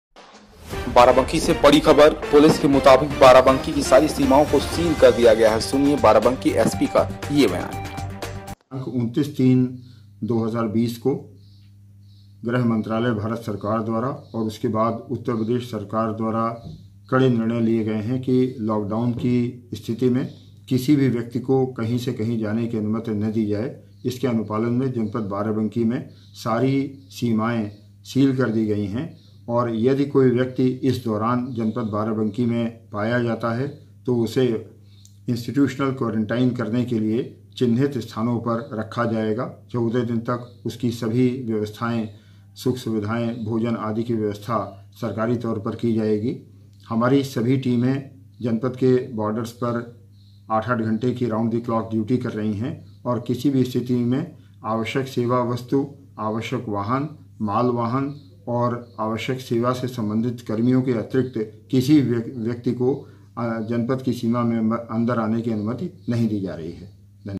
बाराबंकी जनपद के सभी सीमाएं सील, कोई न बाहर जा सकता है न कोई अंदर आ सकता है : पुलिस कप्तान
गृहमंत्रालय व प्रदेश सरकार के आदेश पर हुआ कार्रवाई#shrinarad media